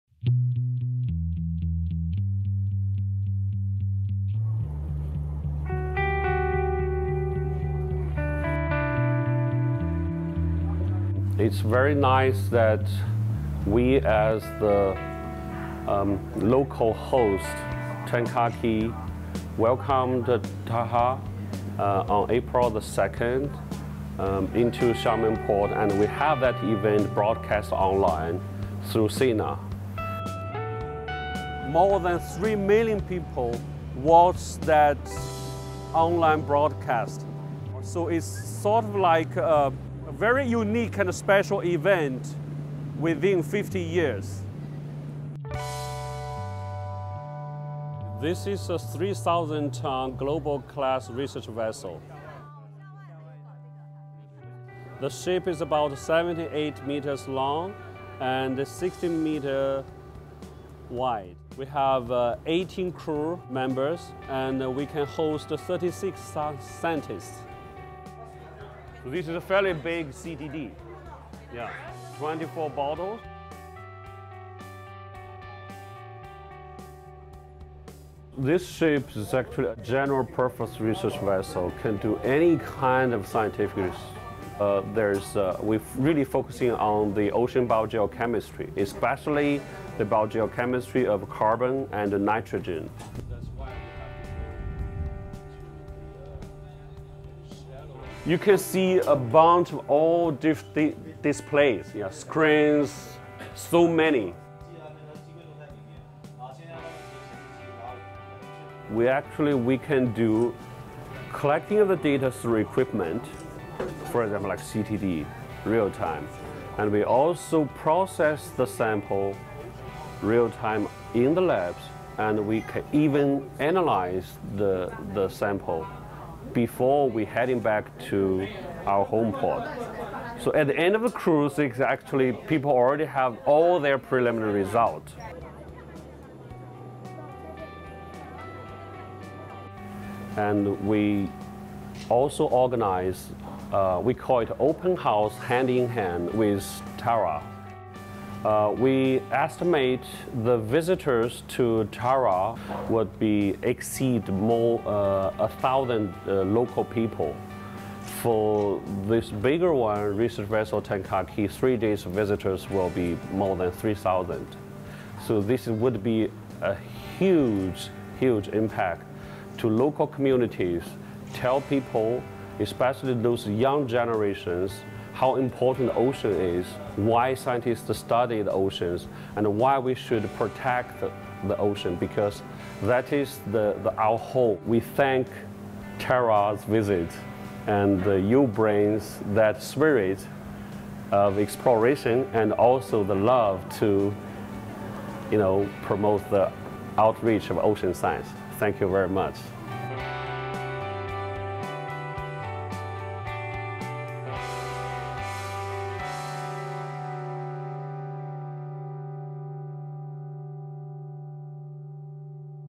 [Tara Pacific] Tara et Tan Kah Kee : missions communes franco-chinoises
Le navire océanographique de l’Université de Xiamen, le Tan Kah Kee, est venu accueillir Tara lors de son arrivée à Xiamen. Et c’est côte à côte que les deux bateaux scientifiques, français et chinois, ont traversé le chenal qui mène jusqu’au port de la ville.
En attendant d’organiser un jour une expédition scientifique commune, Tara et le Tan Kah Kee ont ouvert leurs portes au public chinois. Près de 4 500 visiteurs ont ainsi pu être sensibilisé aux enjeux de la protection de Océans.
[Tara Pacific ] Tara and Tan Kah Kee: Franco-Chinese joint missions
The oceanographic research vessel of Xiamen University, Tan Kah Kee, welcomed Tara upon her arrival in Xiamen. Side by side, these two scientific research vessels, French and Chinese, crossed the channel leading to the city's harbor.
Pending a future joint scientific expedition, Tara and the Tan Kah Kee welcomed aboard Chinese visitors. Nearly 4,500 people were thus made aware of the challenges involved in protecting the Oceans.

© Noémie Olive - Fondation Tara Expeditions